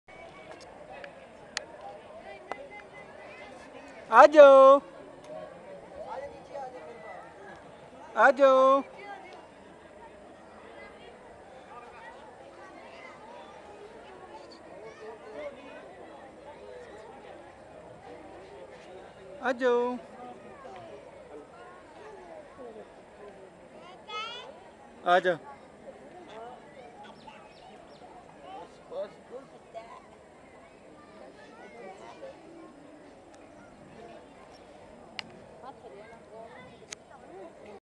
RANA AAYAN MUSTAFA (Sahiwal Distt. Sargodha)
Rana Aayan Mustafa,

Stairs of Faisal Masjid

10 months old baby, June-2009